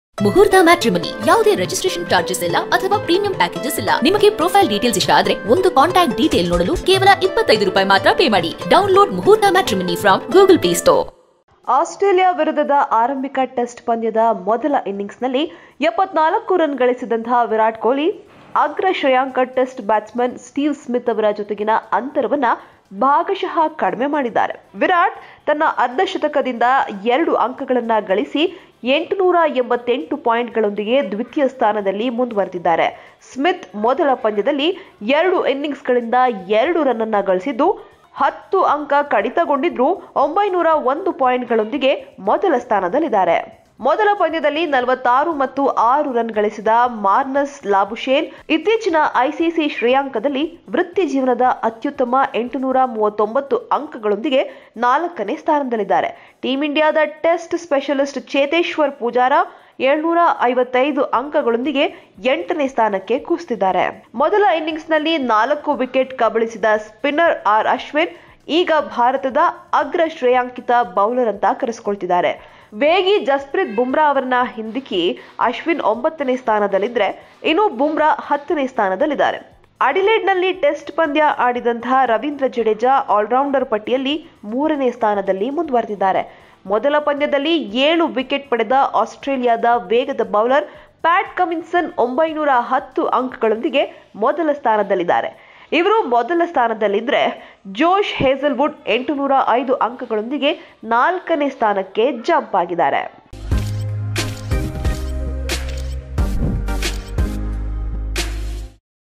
ICC Ranking ನಲ್ಲಿ ಆಟಗಾರರ Ratings ನೋಡಿ!! | Oneindia Kannada
ಕೊಹ್ಲಿ ತನ್ನ ಅರ್ಧಶತಕದಿಂದ ಎರಡು ಅಂಕಗಳನ್ನು ಗಳಿಸಿ 888 ಪಾಯಿಂಟ್​ಗಳೊಂದಿಗೆ ದ್ವಿತೀಯ ಸ್ಥಾನದಲ್ಲಿ ಮುಂದುವರೆದಿದ್ದಾರೆ.

Kohli leads the team in scoring with two fifties and 888 points. where Ashwin and Burmah are in close competition